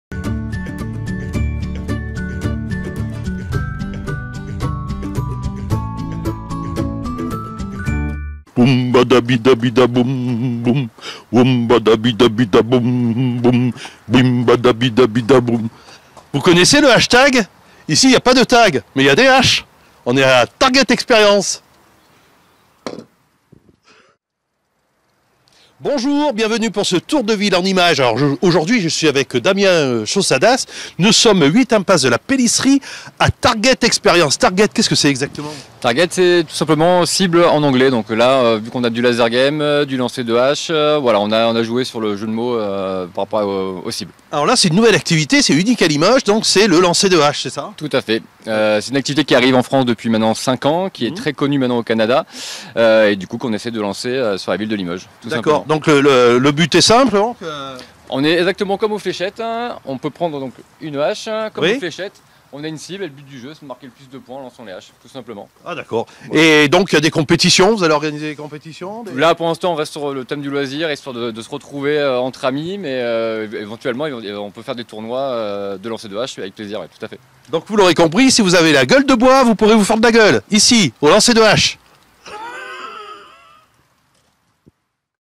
Tour de ville - Chez Target Experience à Limoges, c'est l'heure hache...
Target expérience, une nouvelle activité au Laser Game Evolution, 8 impasse de la Pelisserie à Limoges (zone Sud).